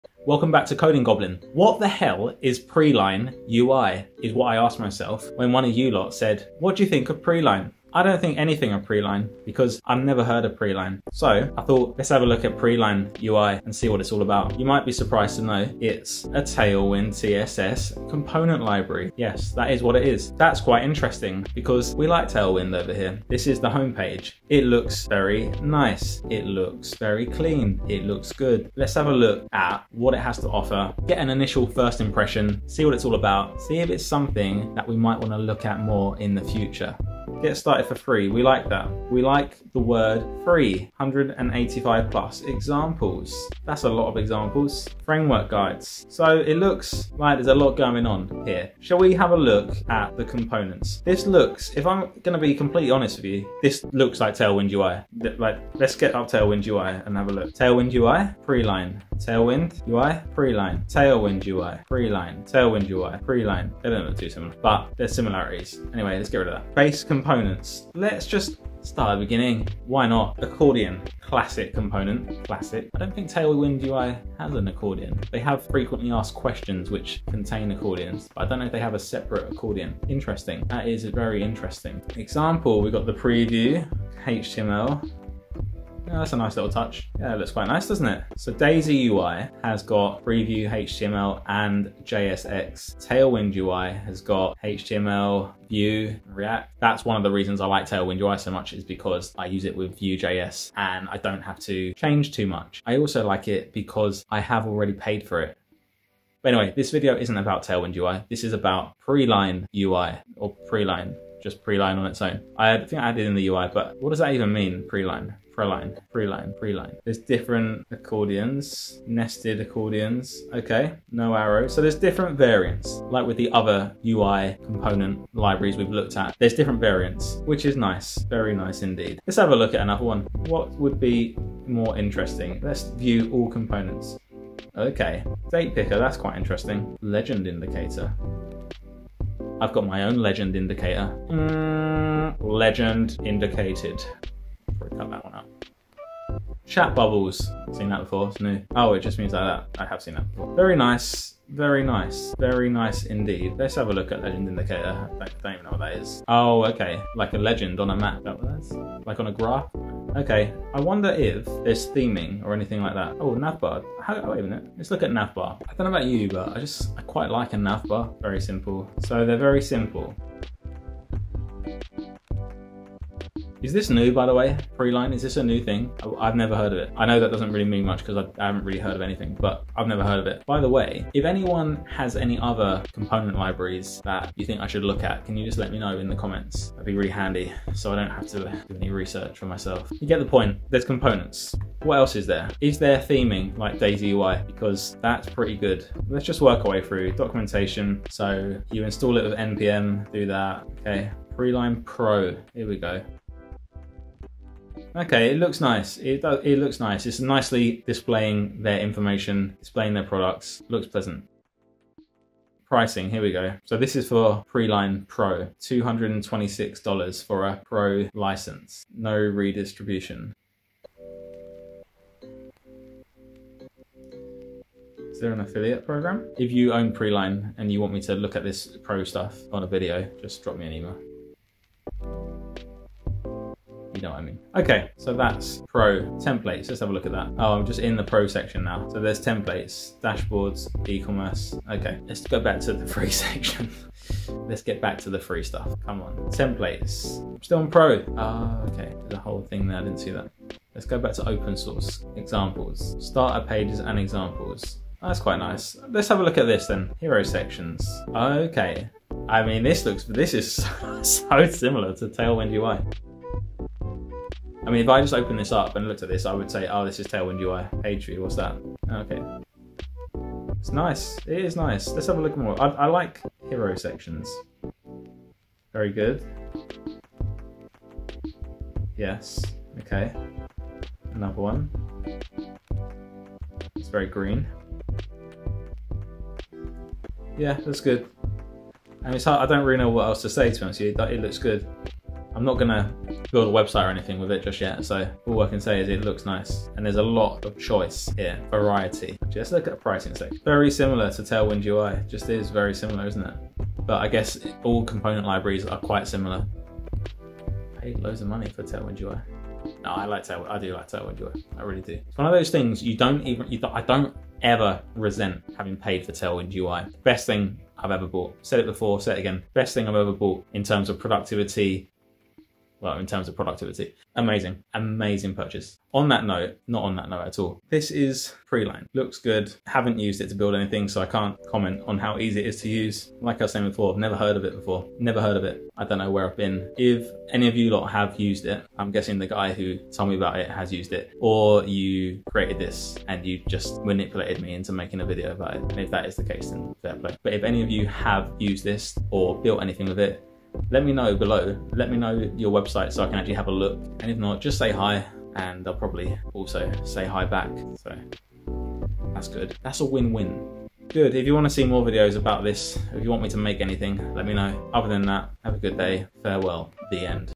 WHAT THE HELL IS PRELINE?
Preline UI is a UI component library built with TailwindCSS. I'd never heard of this Tailwind CSS component library before so I though I would take a look at it. Preline UI seems very clean and the components look great. It's no surprise, seeing how great TailwindCSS is.

Take a look and check it out for yourself. I've not yet built anything with Preline UI but it looks very promising and I may well build a website with it in the future.